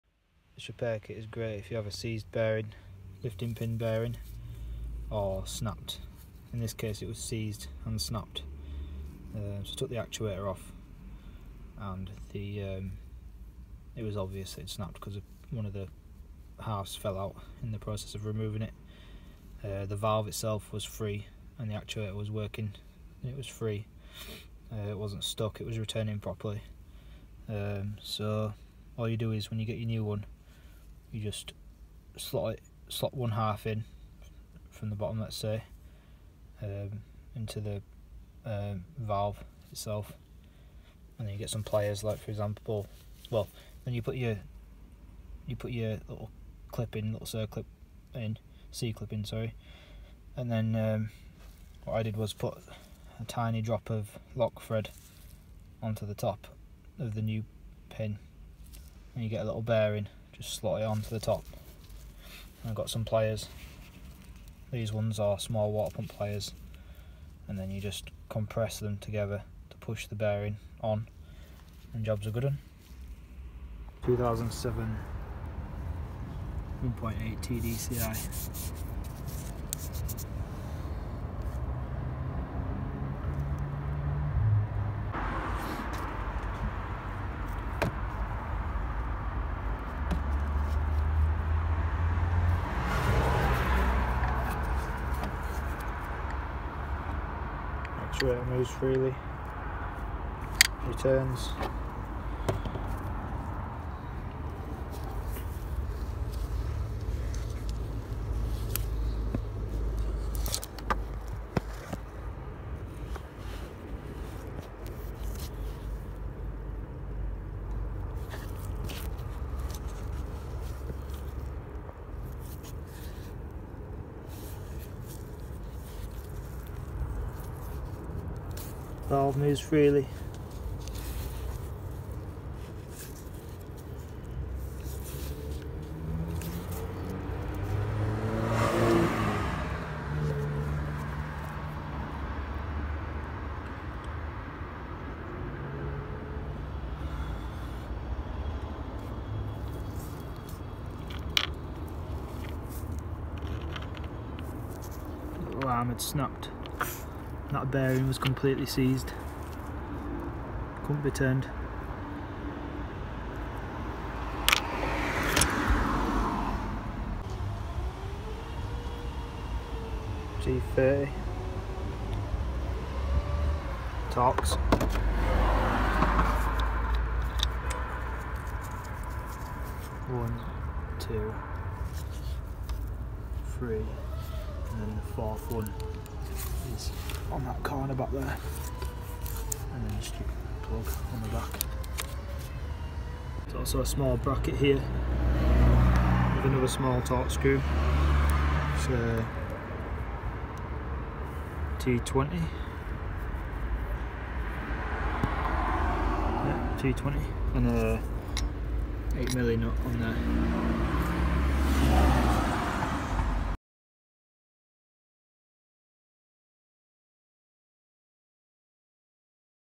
Ford 1.8tdci EGR fault repaired using lifting pin repair kit
2007 Ford S-max 1.8tdci DuraTorq

Primary Symptoms:
Random engine stalling/cutting out
Black smoking
Loss of power
EML light

Breakdown service scanned for fault codes and produced an apparent EGR code, erased the memory, the fault persisted but the code was not presented to us and the OBD memory was clear when the vehicle arrived.

After repairing the EGR valve the vehicle drove faultlessly. All the work was done with the inlet manifold still attached.

If your 1.8tdci is suffering with EGR faults I’d recommend carefully removing the EGR actuator and examining the EGR valve to see whether the valve and actuator are seized, if the actuator is not returning to its position when turned or if the lifting pin/bearings between the actuator and valve are broken.

EGR valves on these engines form part of the inlet manifold and it’s a relatively time consuming and expensive job to replace it, When it may be possible to repair your existing EGR valve for a fraction of the cost and effort.

 Here’s a link to the lifting kit: